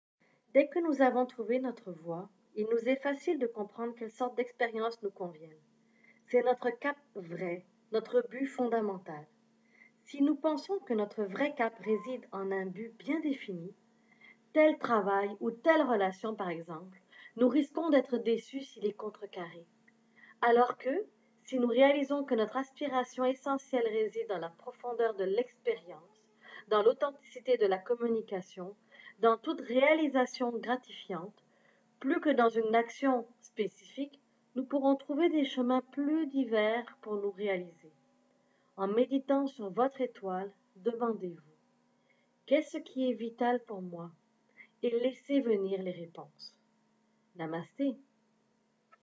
Cap vrai - #23
MÉDITATION : CAP VRAI, Cette méditation a été tirée du livre 1001 Méditations, Comment trouver la paix intérieur de Mike George.

Zarah Issany
Coach et conseillère en développement de carrière et relation d'aide